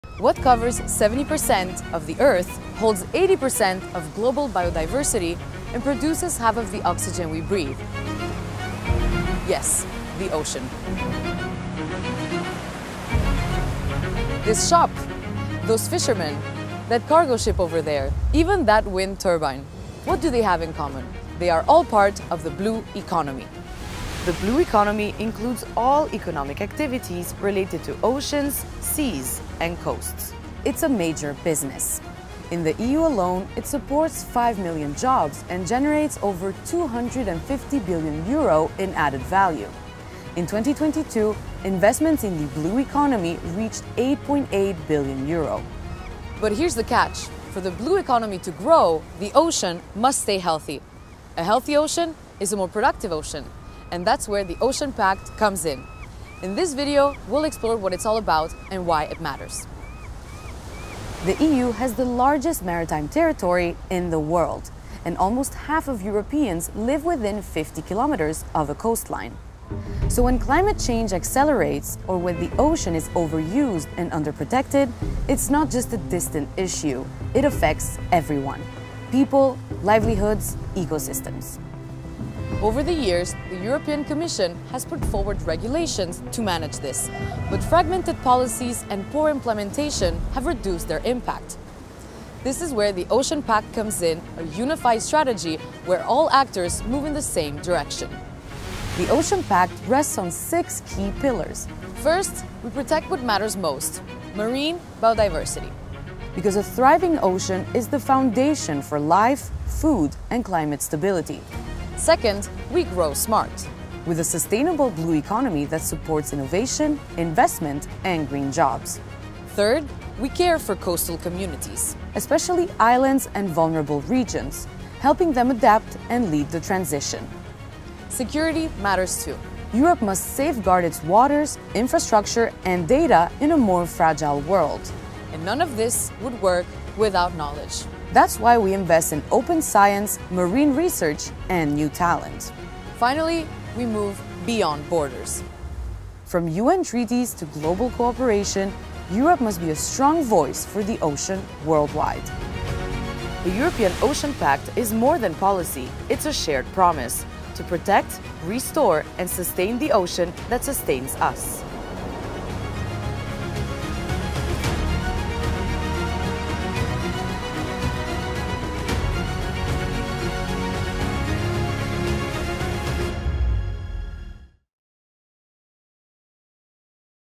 What Is the European Ocean Pact? And why is it necessary?
The ocean powers life on Earth. It covers 70% of the planet, produces half our oxygen, and holds 80% of global biodiversity. But it's at a tipping point.

To protect this vital resource while unlocking its full potential, the EU launched the European Ocean Pact, a unified strategy to shape the future of ocean governance.

From fisheries to offshore wind. From biodiversity to marine research. From local communities to global cooperation. One Ocean, One strategy.

00:00 How Oceans are essential
00:24 EU Blue Economy
00:56 Why does the European Ocean Pact matter?
01:31 The 6th pillar of the European Ocean Pact